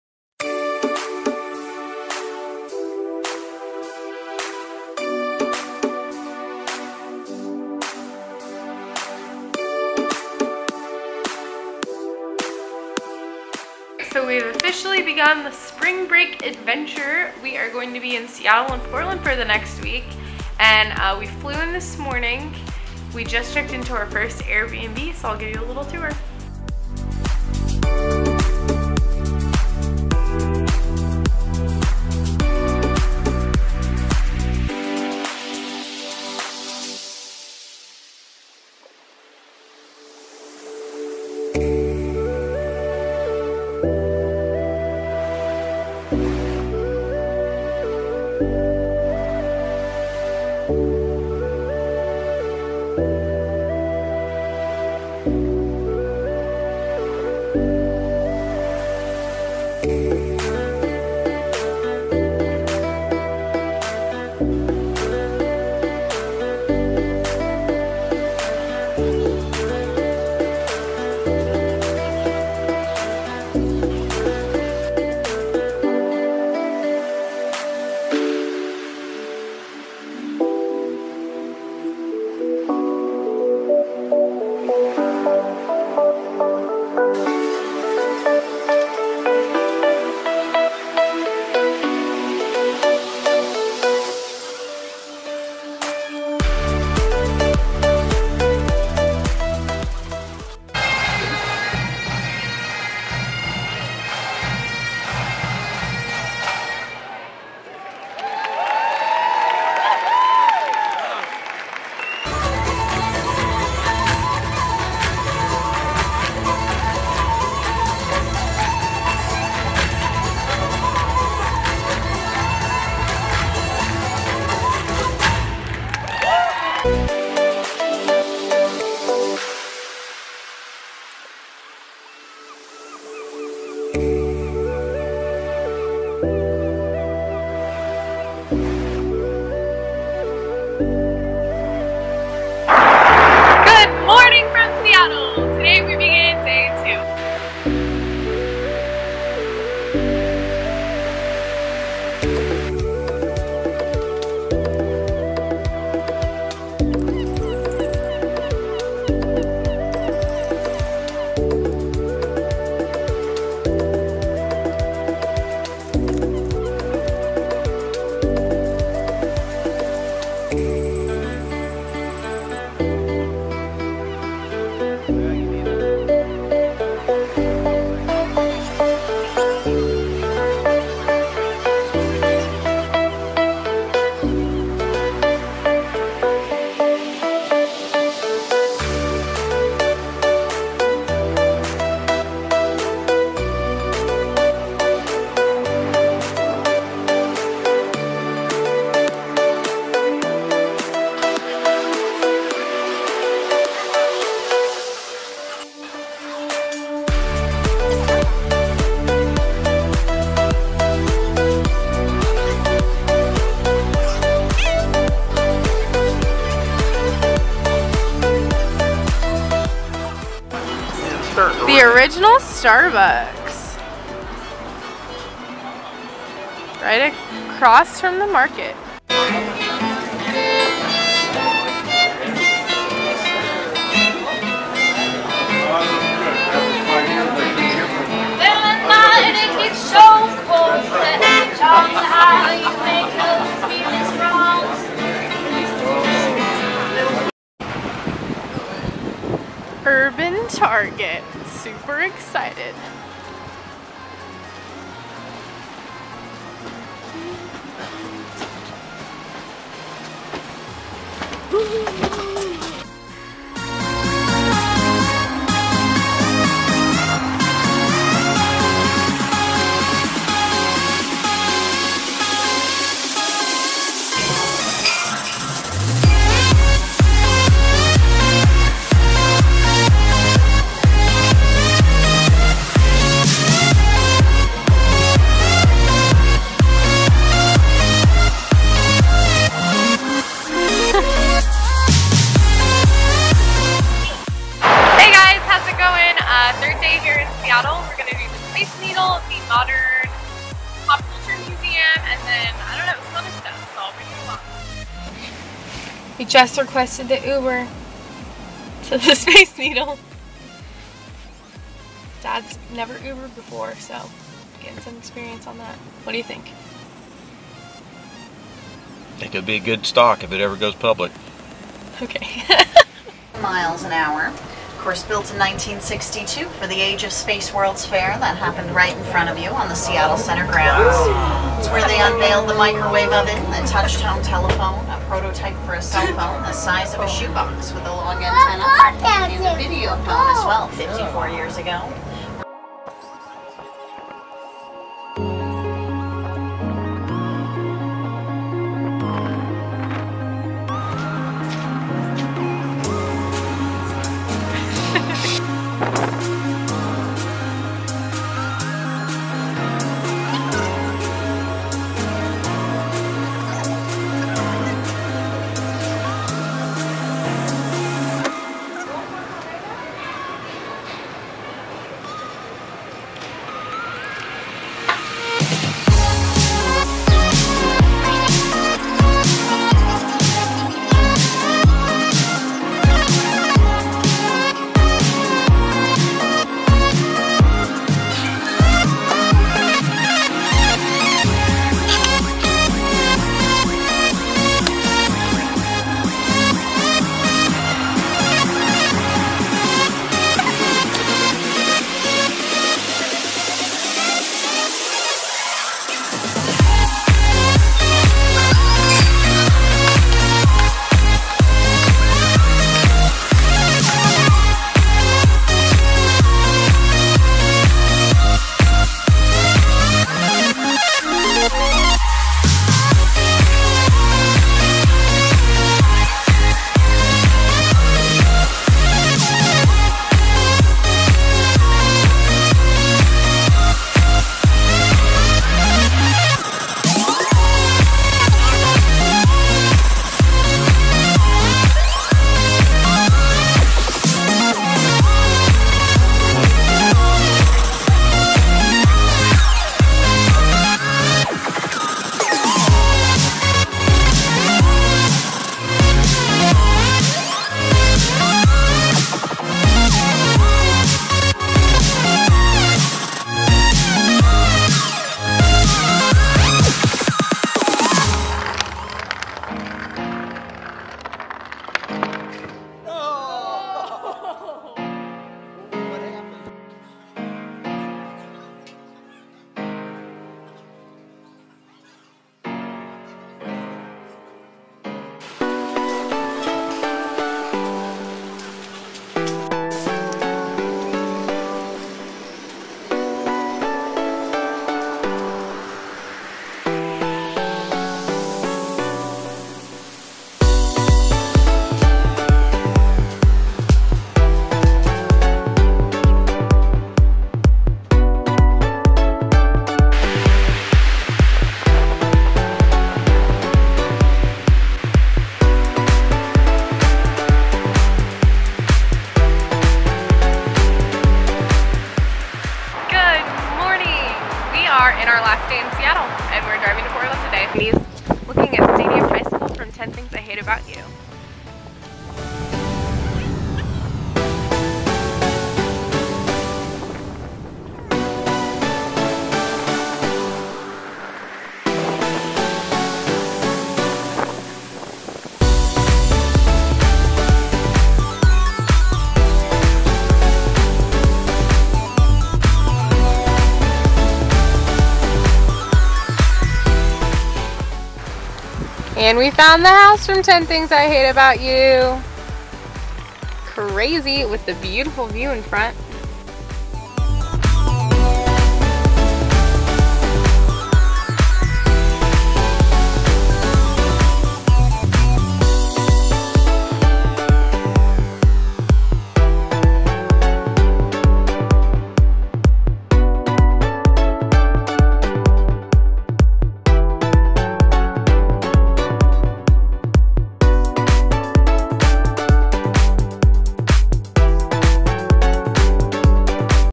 Exploring Seattle | March 2017 | The Opinionated Lassie
Follow me and my father around the beautiful city of Seattle, WA! Video includes Starbucks Reserve, Seattle Center, The Space Needle, Museum of Pop Culture, and Stadium High School!